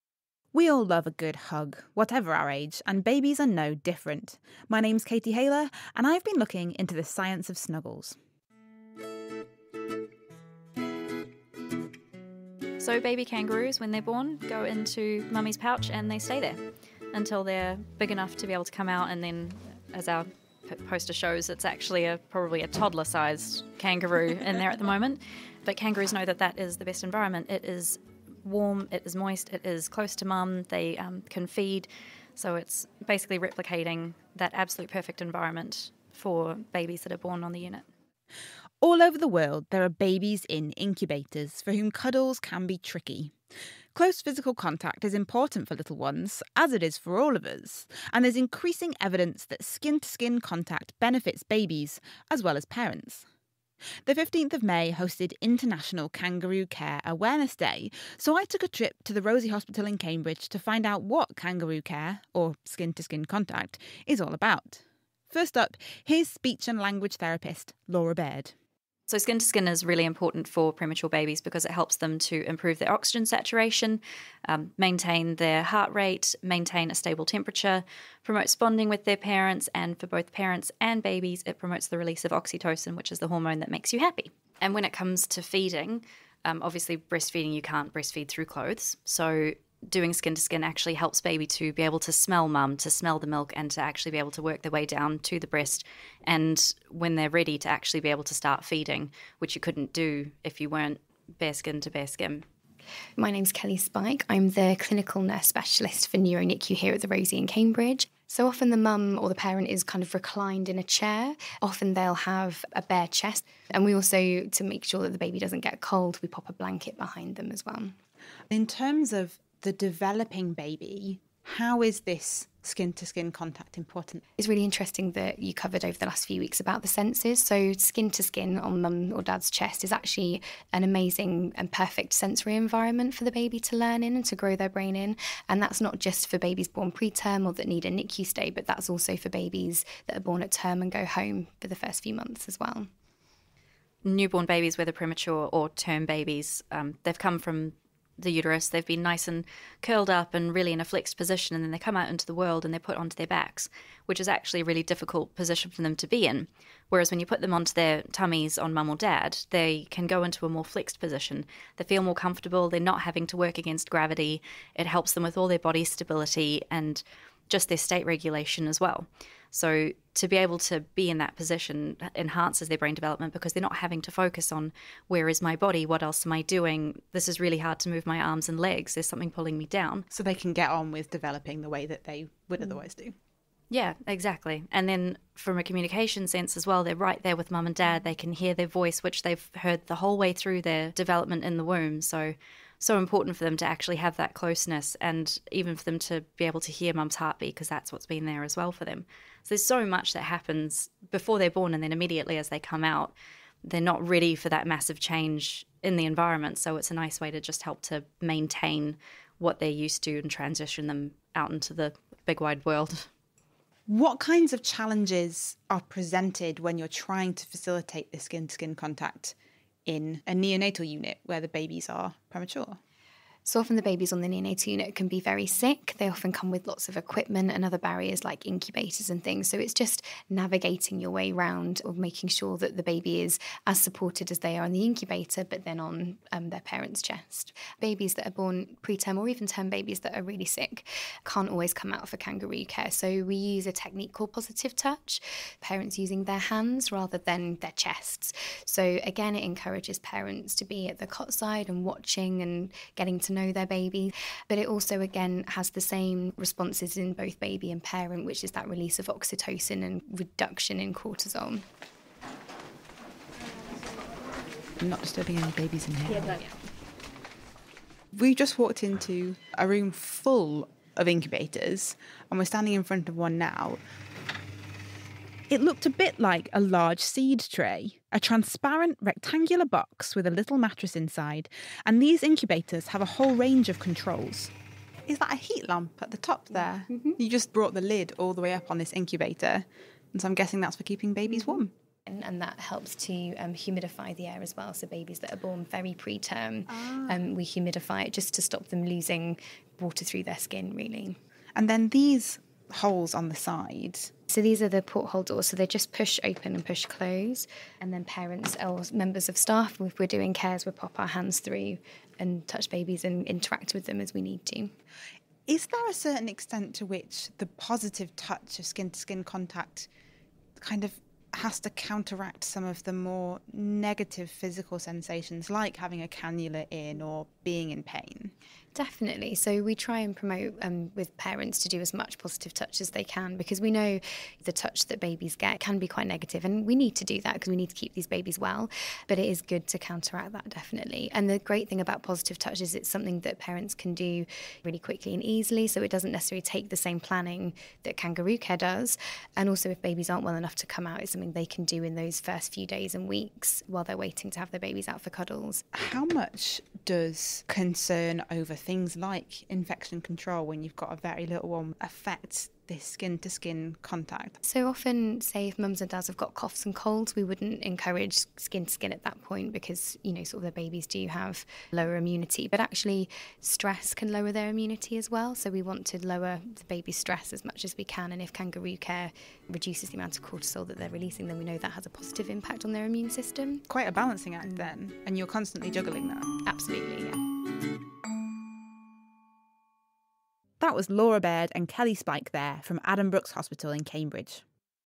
Kangaroo care: the science of snuggles
We all love a good hug, whatever our age! Babies are no different. There's growing evidence that skin-to-skin contact is beneficial for babies, so Katie Haylor spoke with Laura Baird and Kelly Spike from Addenbrookes Hospital in Cambridge to find out what it's all about... Like this podcast? Please help us by supporting the Naked Scientists